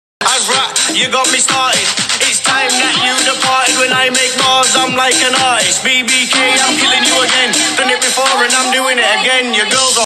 Emu kill u
Ye c this emu , hunts the little creepers down an eats them